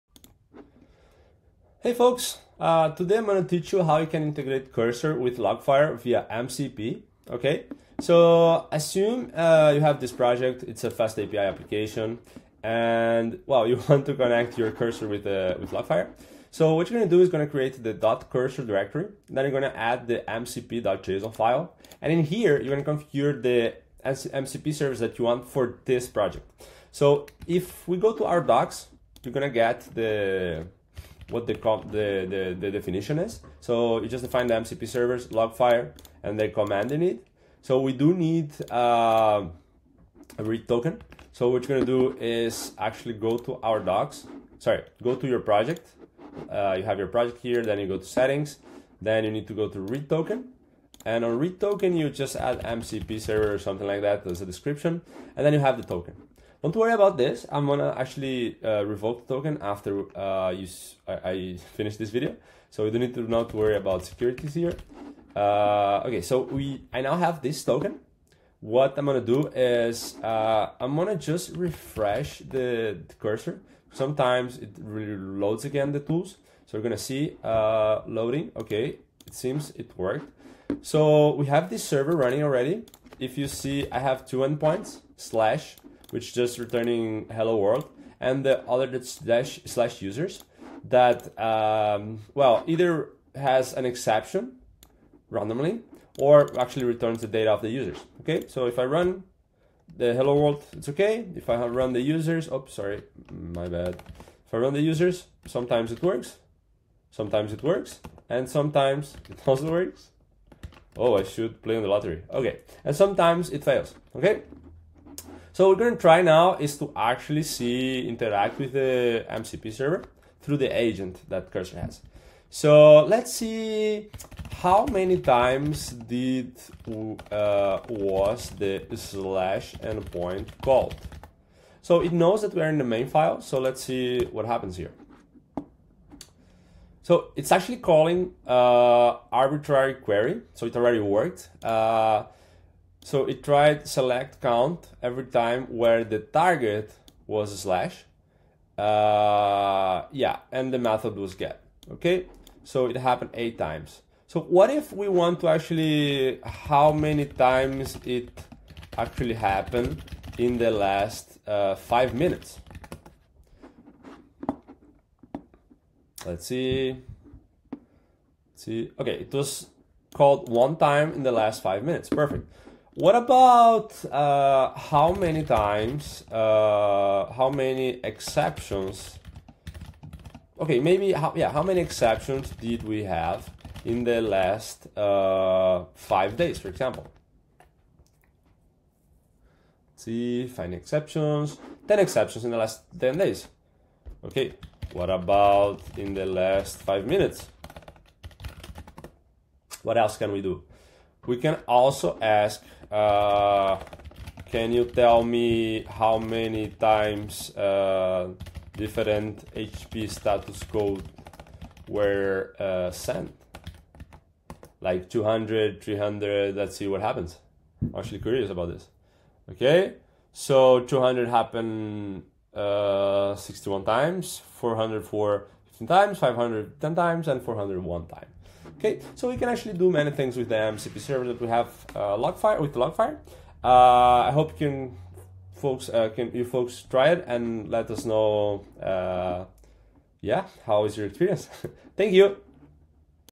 Cursor with Logfire MCP
🔥 Level Up Your Development Workflow with Cursor AI + Logfire Observability!

In this video, I'll show you how to supercharge your coding experience by integrating Cursor AI with Logfire's MCP (Model Context Protocol) for seamless observability and
debugging.